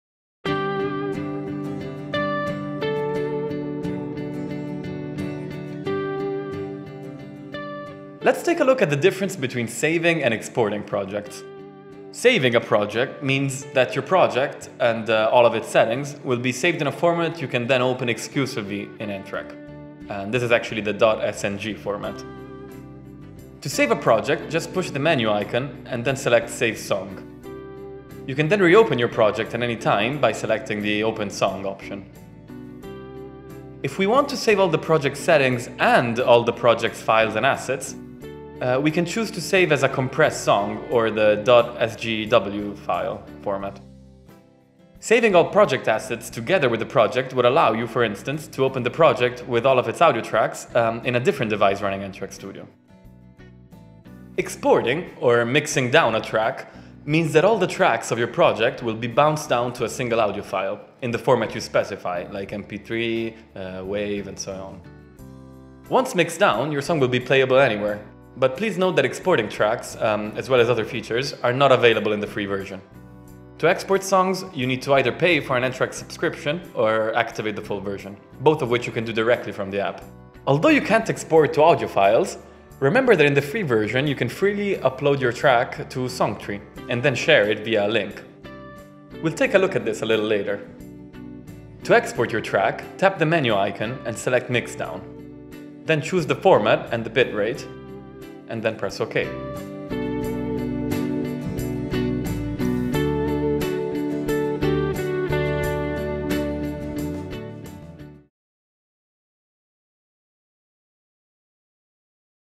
Saving and exporting your song || n-Track Studio Android Tutorial Series (Beginners)
In this tutorial we'll look at saving and exporting your n-Track Studio projects. We'll clarify the difference between saving and mixing down a project, and the various options you have while using the free or paid version of the app.

n-Track Studio is a multitrack recording studio and digital audio workstation app for iOS, Android, Mac and PC.